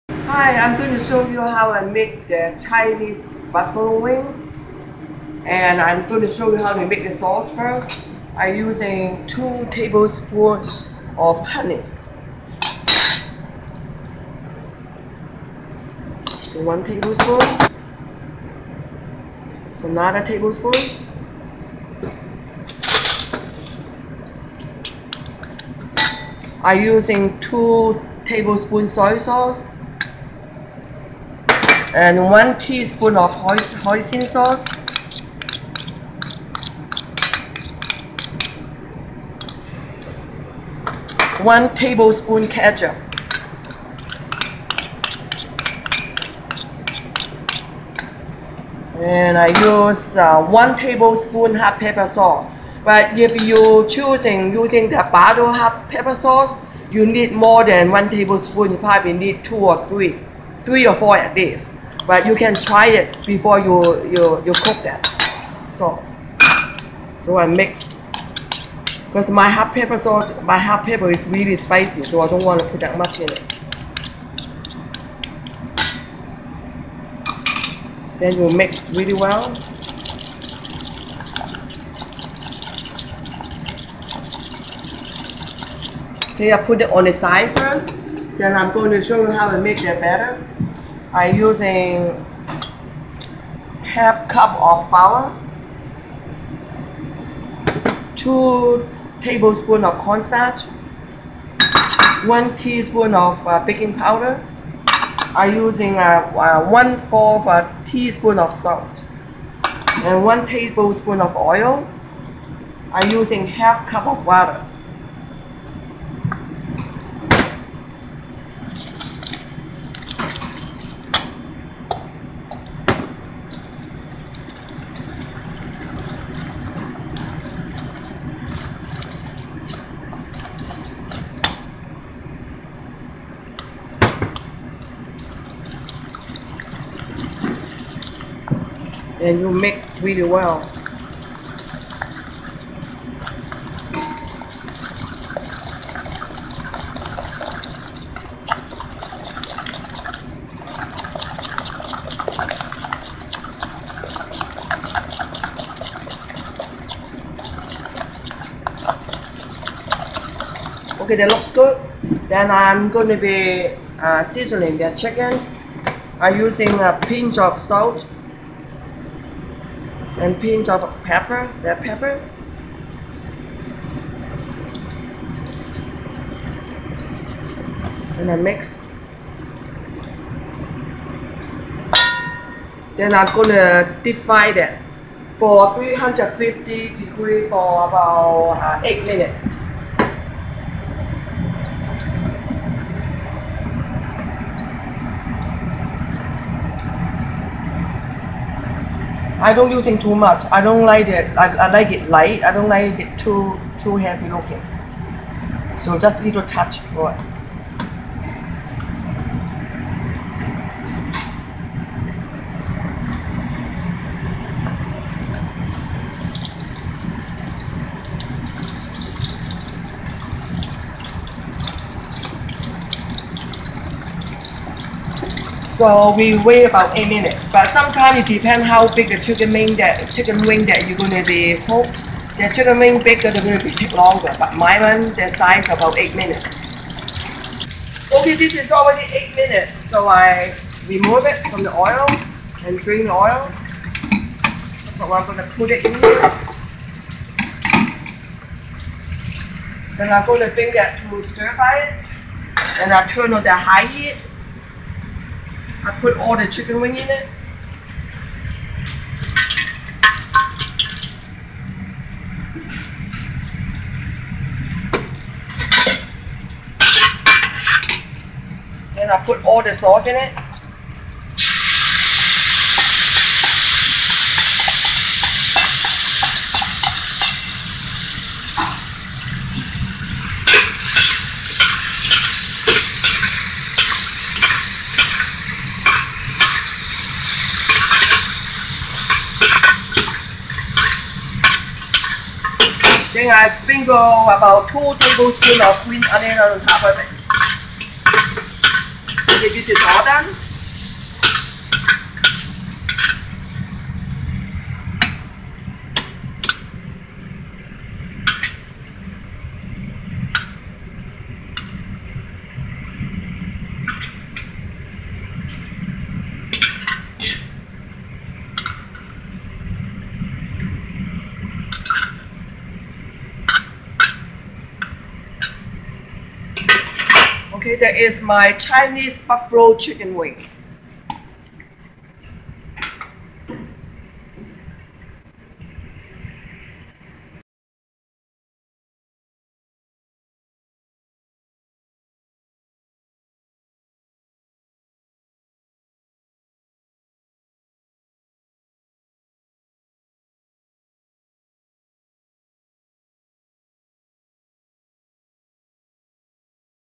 Chinese Buffalo Wings
Learn How to Make Chinese Buffalo Chicken Wings